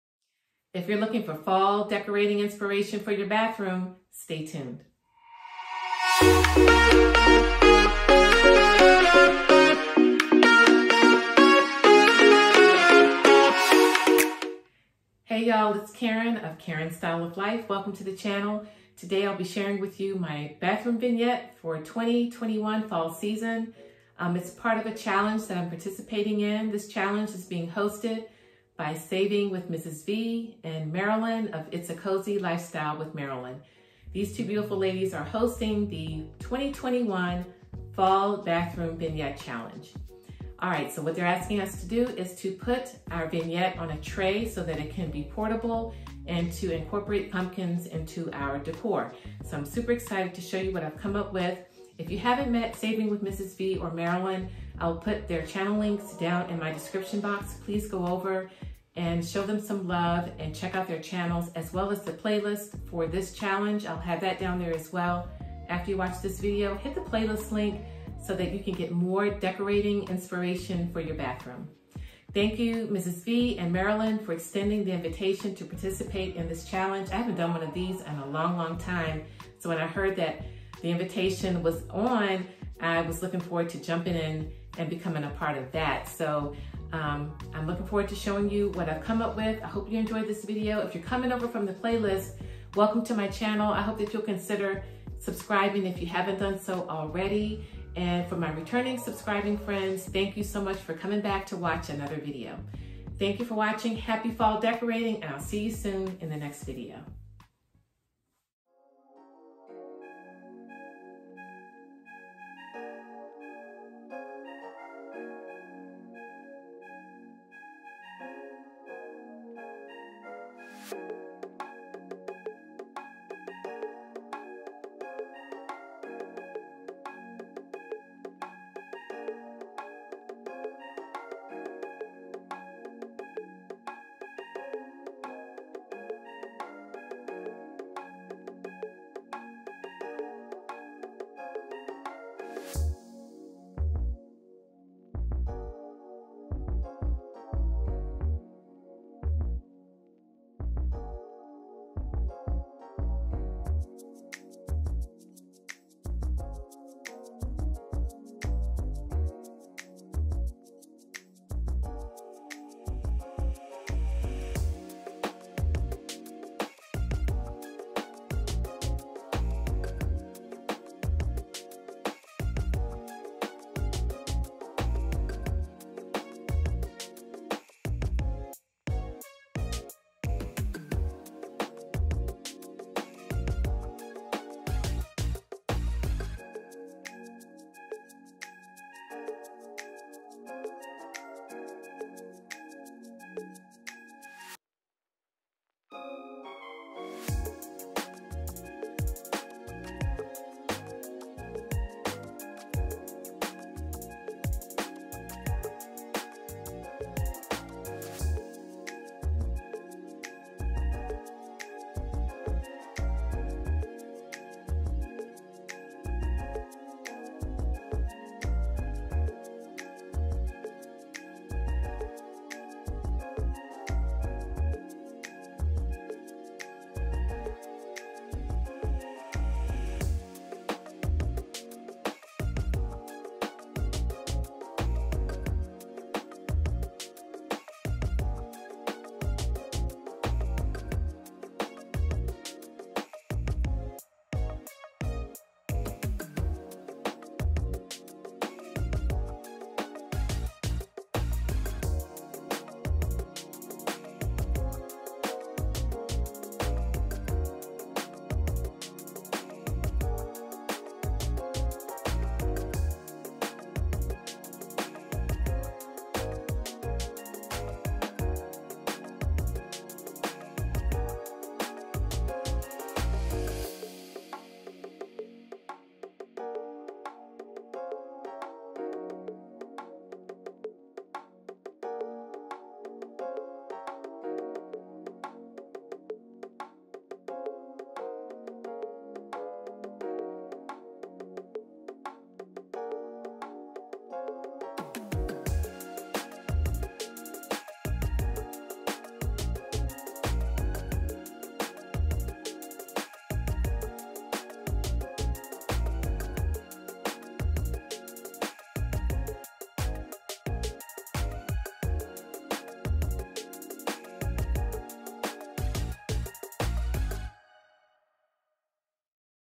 Fall Bathroom Vignette Challenge 2021 | Functional Bathroom Decor
Happy Fall Y'all!!  In this video I'm participating in the Fall Bathroom Vignette Challenge for 2021.  Join me as I share how I've decorated my master bathroom for the Fall season, but still keeping it functional.  Please also check out the playlist below as well as the channels of our lovely hosts.